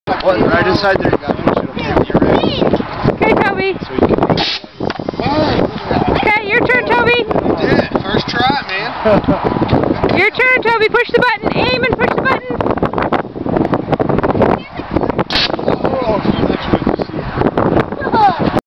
Toby shooting the Corn Cannon at Harvest Farm
Toby shooting the Corn Cannon at Harvest Farm in Wellington, Colorado on October 25, 2009.  Both he and Ben loved this.  They each did it 3 times.  There were targets set up in front of the cannons for them to aim at.  Toby liked this but he was scared of the Pumpkin Blaster.  This was perfect for him.